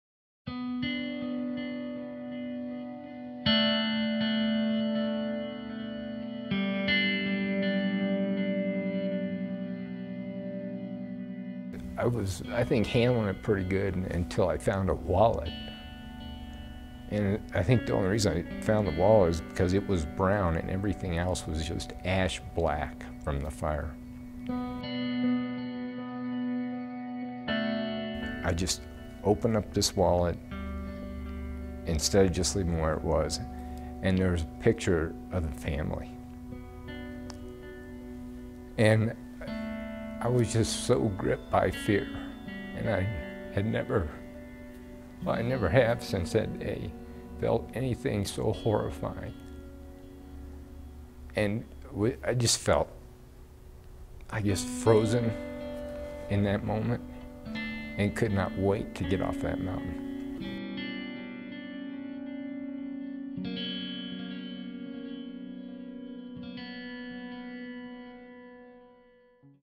Teaser #1 - Black & Gold: Remembering the WSU Plane Crash
John Putt was 12 years old when he joined the Alpine Rescue Team on a recovery mission at the Wichita State University plane crash site. John has never spoken about his experience on the mountain that day, 39 years ago, until now.